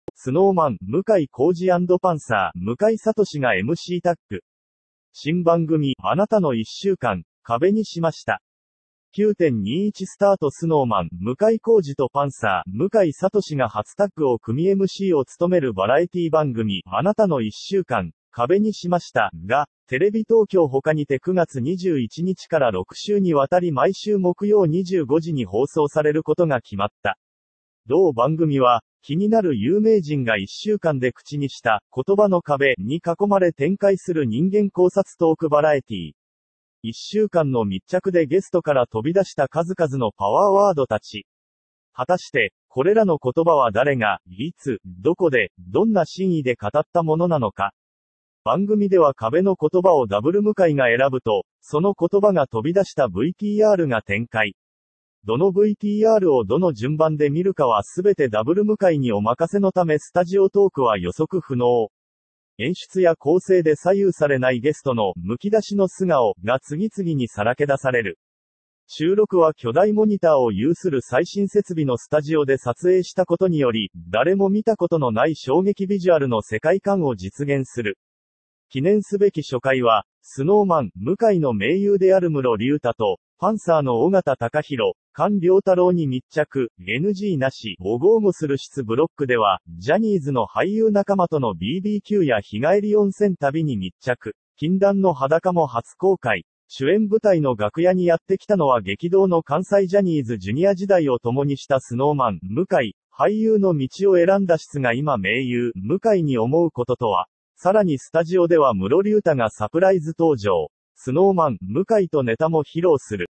Snow Man・向井康二＆パンサー・向井慧がMCタッグ！ 新番組『あなたの1週間、壁にしました』9.21スタート
Snow Man・向井康二とパンサー・向井慧が初タッグを組みMCを務めるバラエティー番組『あなたの1週間、壁にしました』が、京ほかにて9月21日から6週にわたり毎週木曜25時に放送されることが決まった。